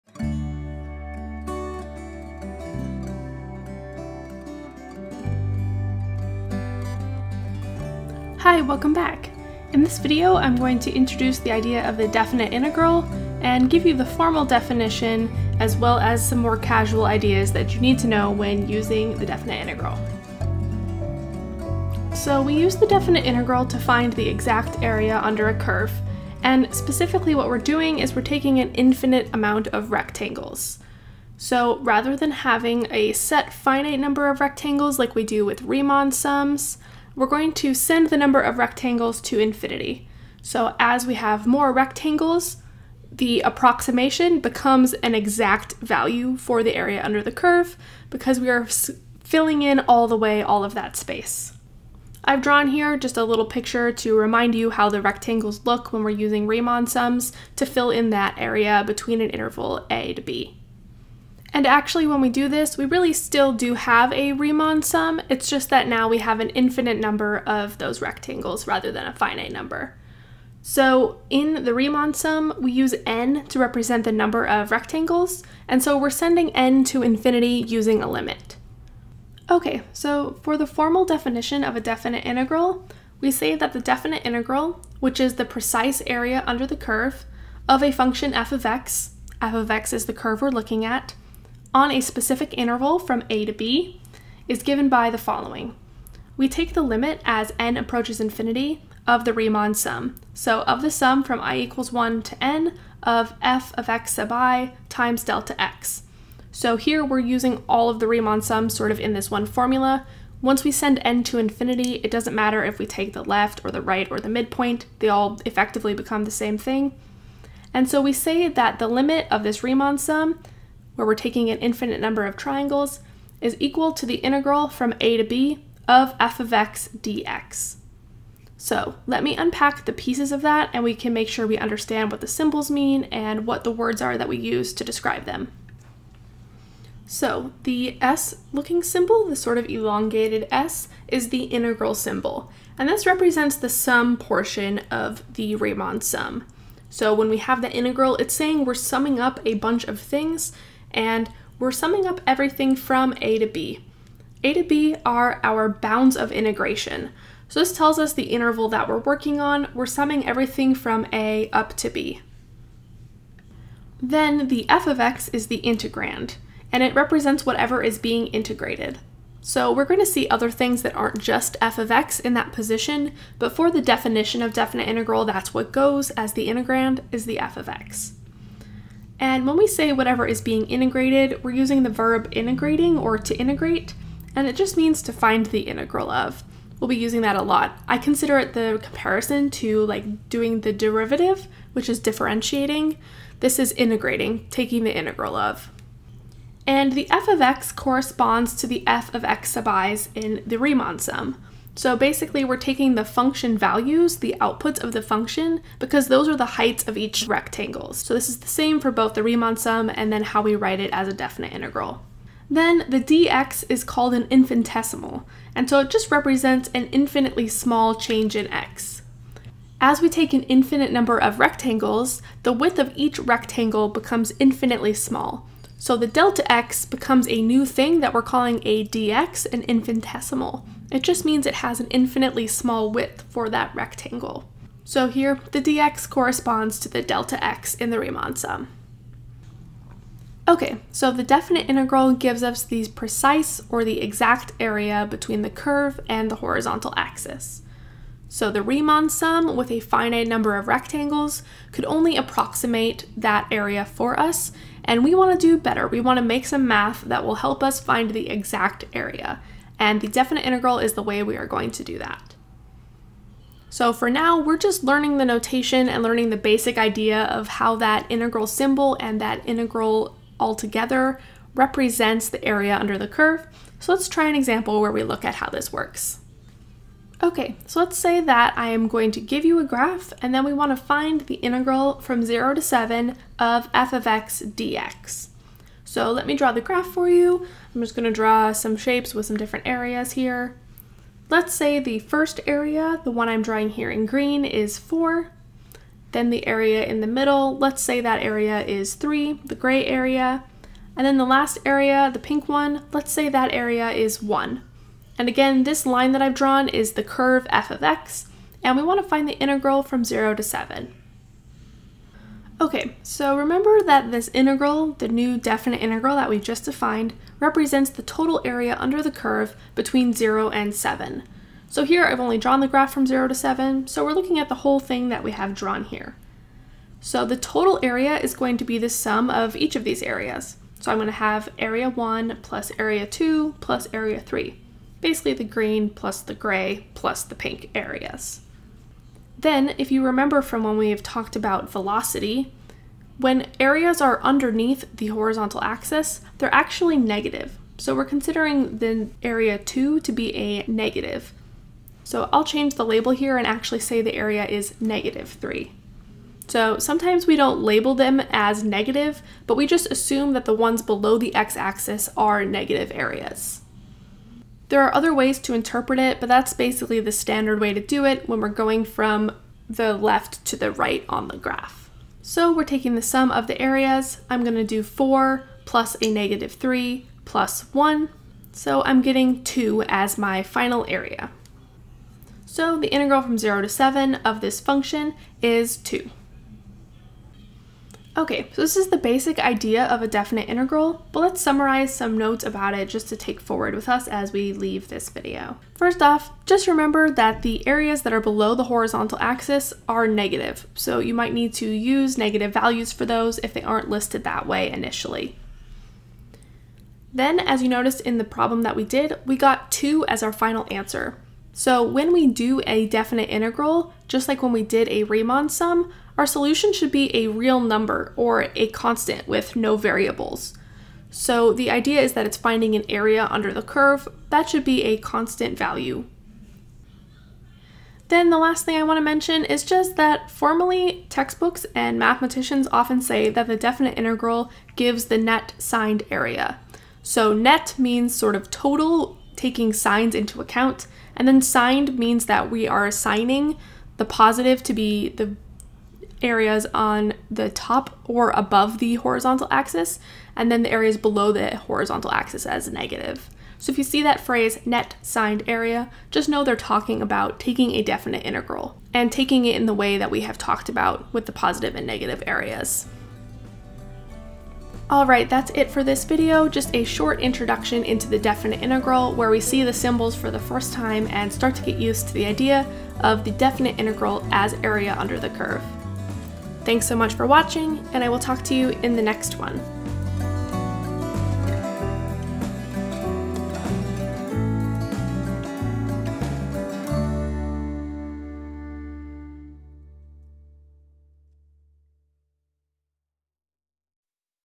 Definition of the Definite Integral
This video defines the definite integral as the precise area between the graph of the function (the curve) and the horizontal axis. This uses Riemann Sums with an infinite number of rectangles.